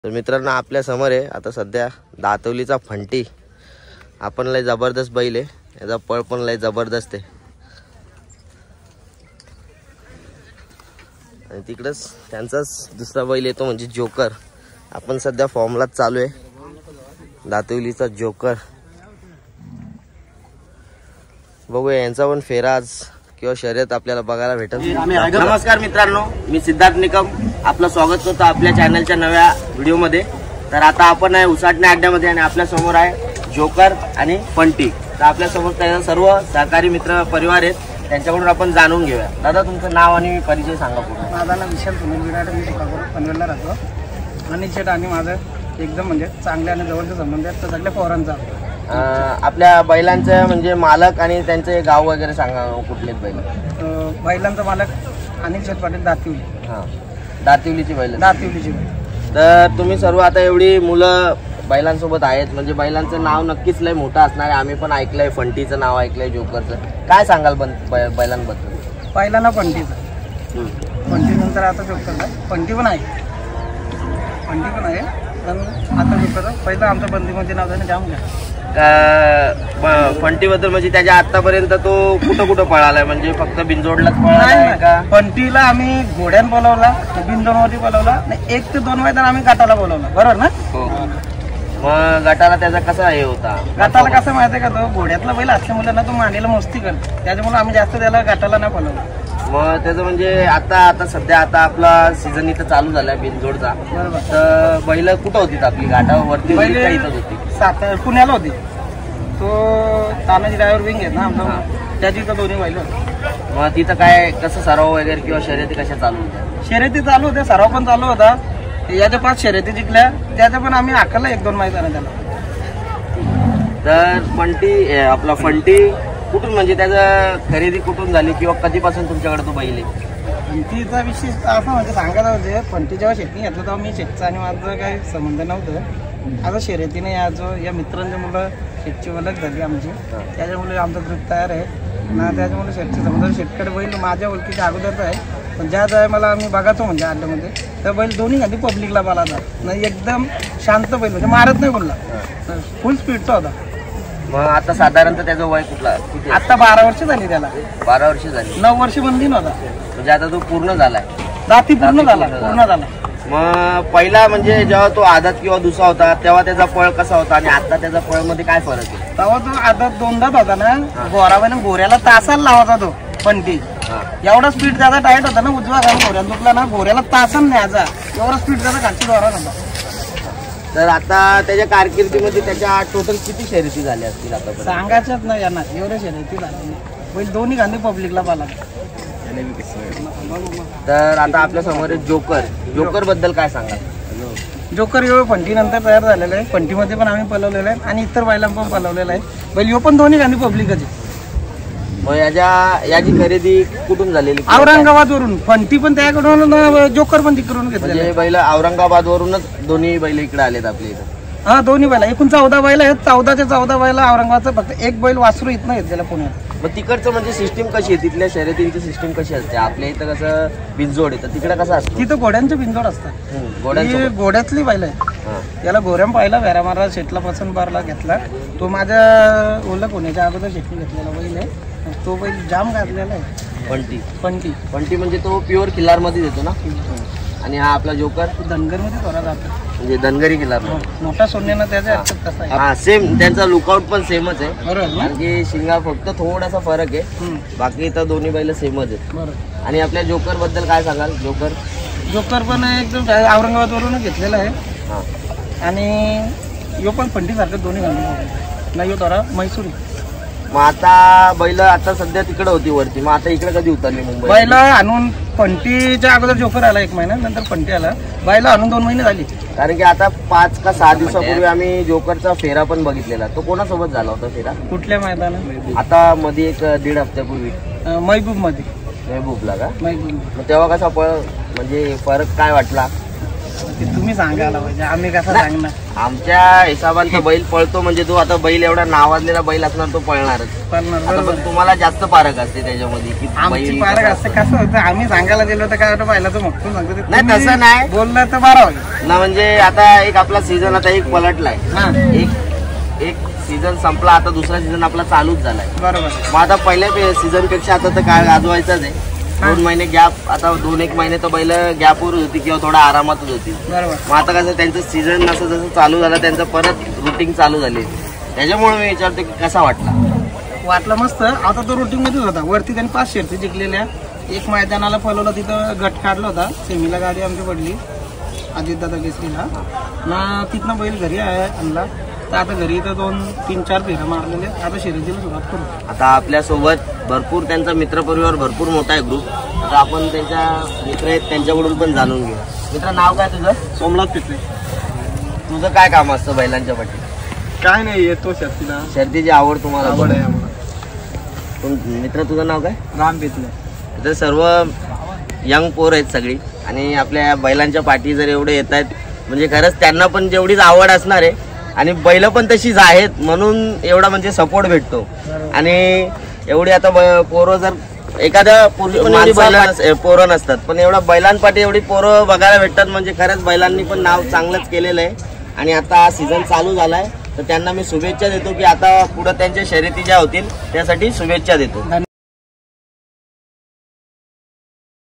मथुर १००१ सोबत फेरा झालेला जोकर ची मुलाखत💯🔥॥bailgadasharyat
अनिल शेठ पाटील (दातीवली) यांचा घरचा साज फंटी & जोकर मुलाखत॥bailgadasharyat